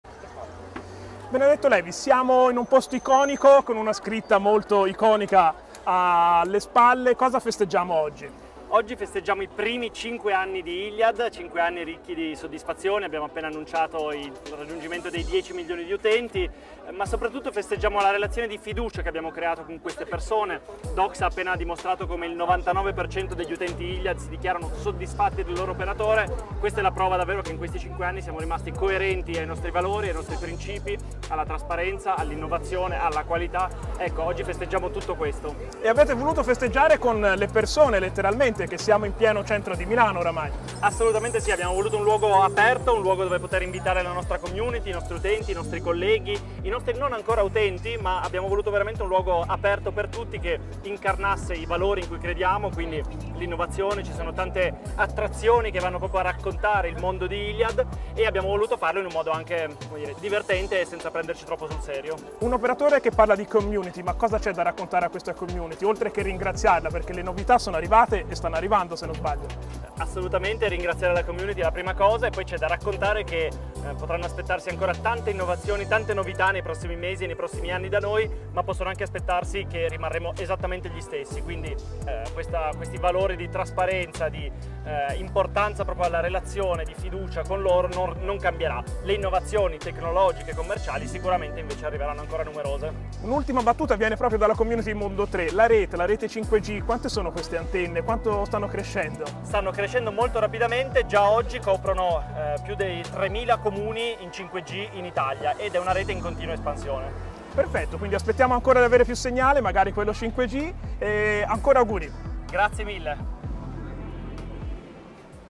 Mondo3 intervista Benedetto Levi (AD iliad) a iliadLand
Benedetto Levi ci introduce al compleanno di @iliaditalia negli spazi di Via Valenza a Milano.